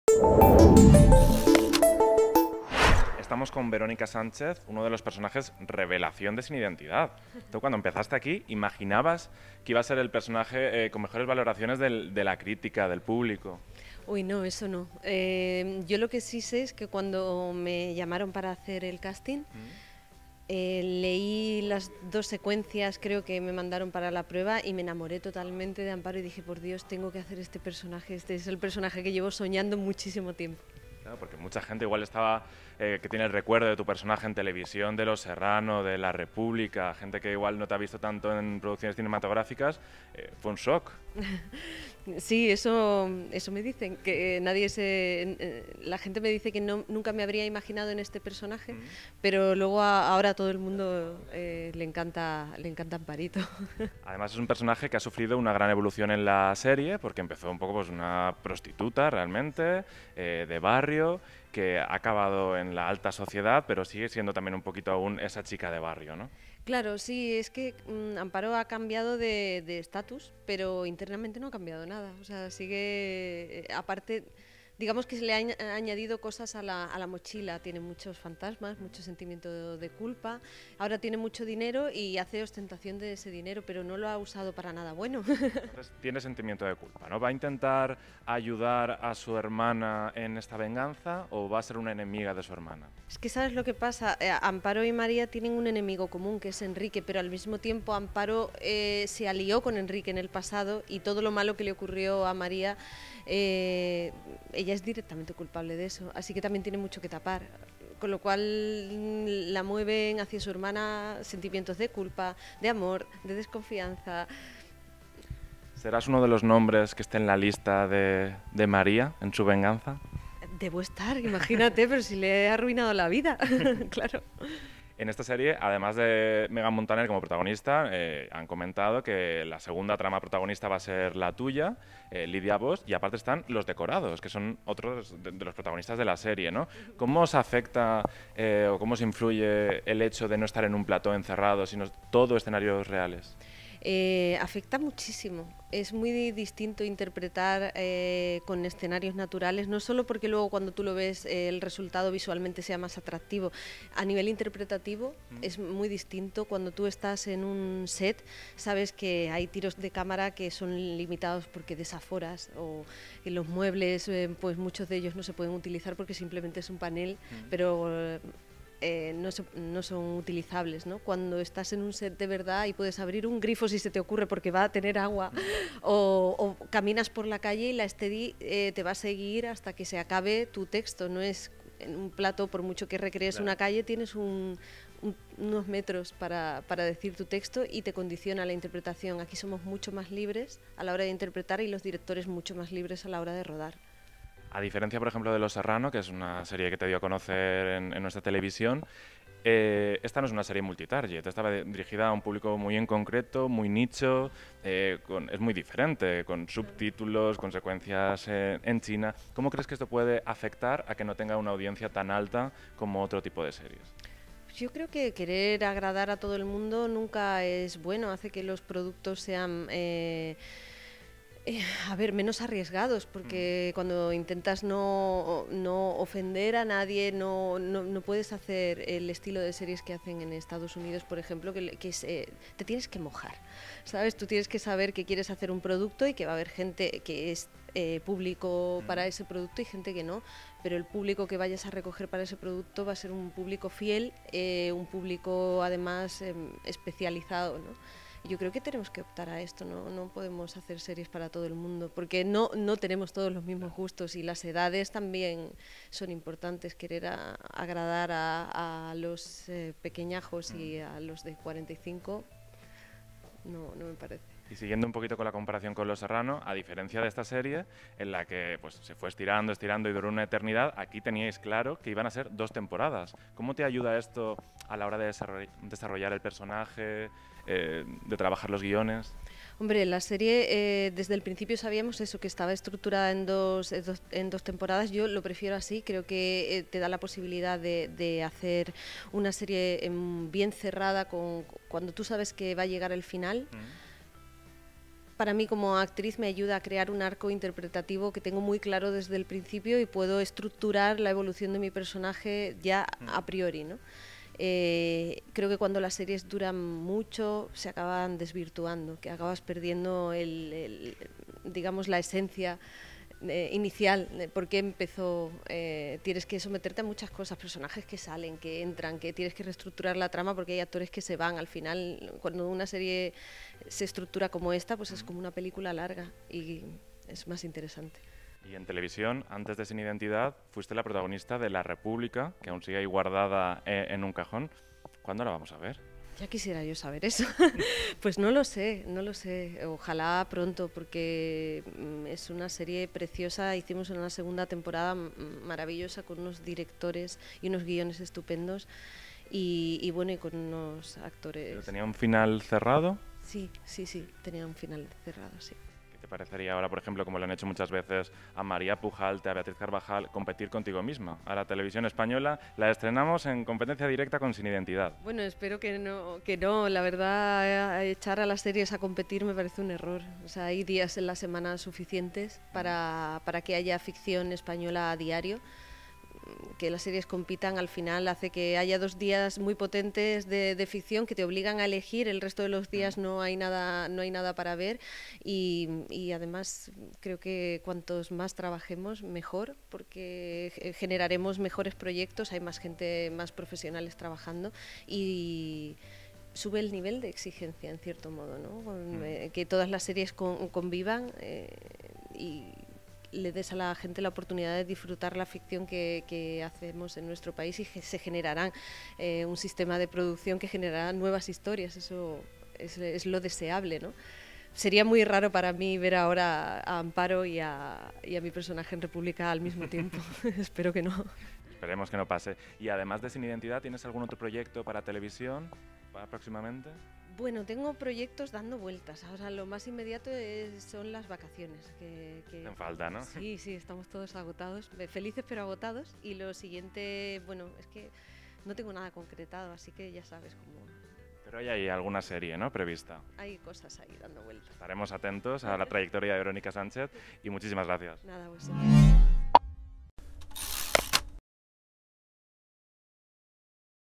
Veronica Sánchez: “Querer agradar a todo el mundo no es bueno, hace que sea menos arriesgado”
La actriz, que da vida a Amparo en la serie de Antena 3 ‘Sin Identidad’, cree que debe estar “en la lista de venganza de María” (Megan Montaner), ya que “le ha arruinado la vida”. “Cuando las series duran mucho se acaban desvirtuando, se pierde la esencia”, explica.